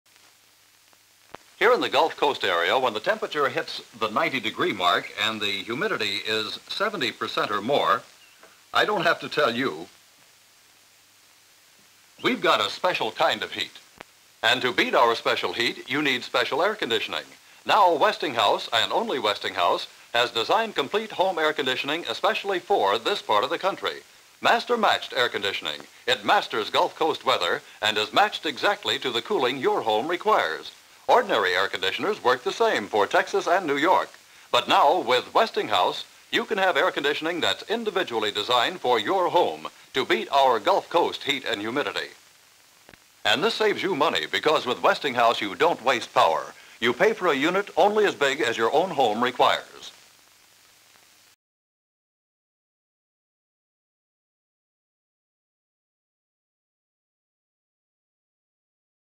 Westinghouse Electric's Air Conditioning Units (1955)
This 1955 advertisement for Westinghouse Electric's Master Matched Air Conditioning units appeals to Gulf Coast residents suffering from extreme heat and humidity. The ad boasts that these AC units master Gulf Coast weather and match the individual cooling needs of your home.